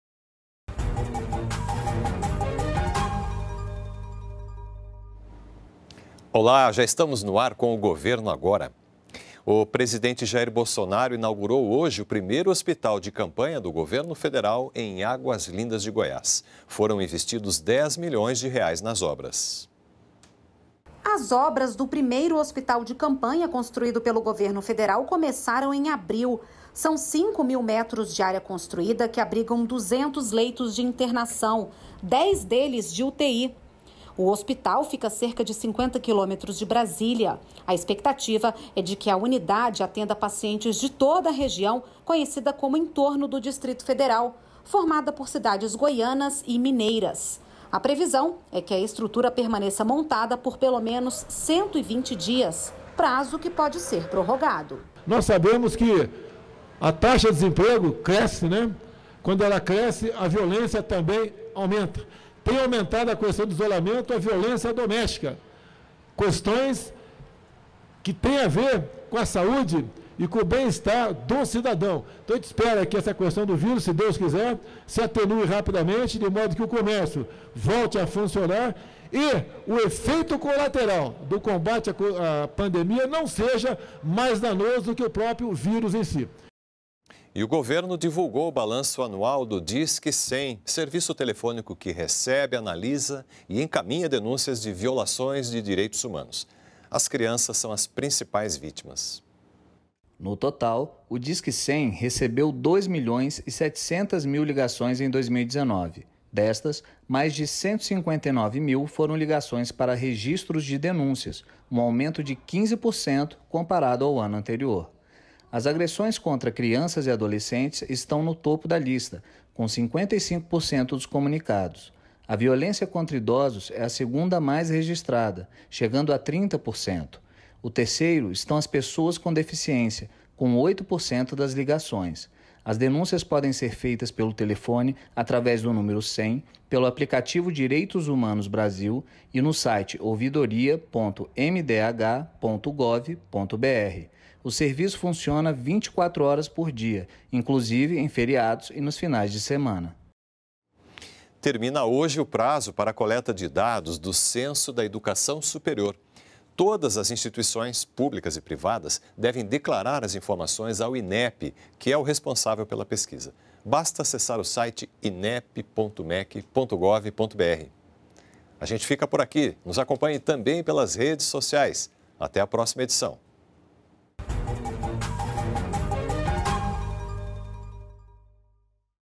Hospital de campanha em Águas Lindas-GO é inaugurado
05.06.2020 - O presidente Jair Bolsonaro inaugurou hoje o primeiro Hospital de Campanha do Governo Federal, em Águas Lindas de Goiás. Veja também: governo divulgou o balanço anual do disque 100, o serviço telefônico de denúncias de violações de direitos humanos.